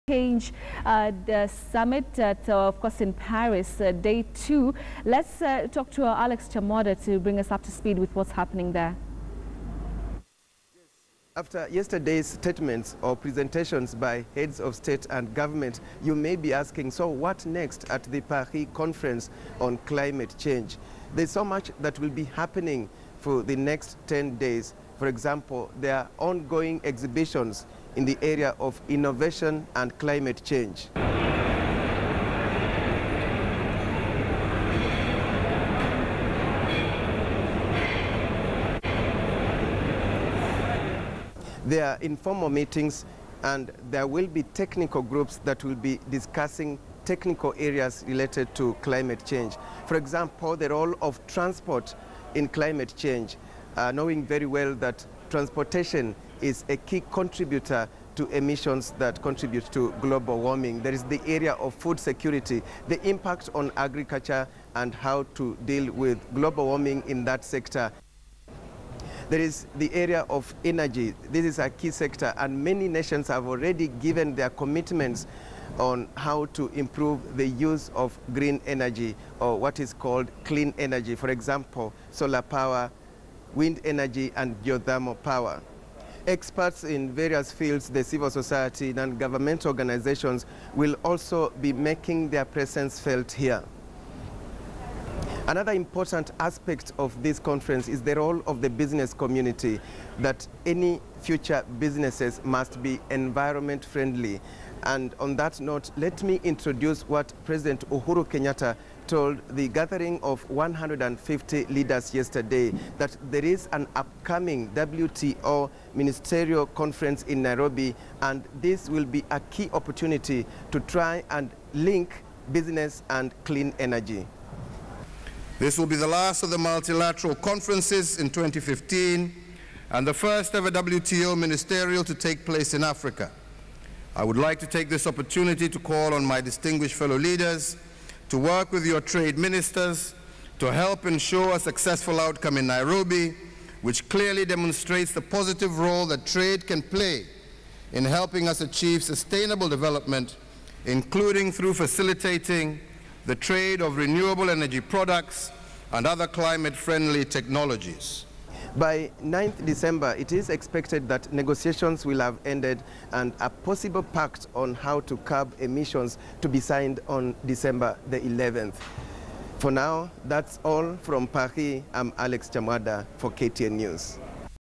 Climate Change Conference in Paris enters crucial stage of technical discussions
Climate Change Conference in Paris now enters crucial stage of technical discussions on issues like energy, transport, and agriculture in relation to global warming. Ten more days to go before the conference ends and as KTN’s special correspondent Alex Chamwada reports president Uhuru Kenyatta says trade and climate change are closely linked and the world should take opportunity of the forthcoming WTO ministerial meeting in Nairobi to discuss that linkage.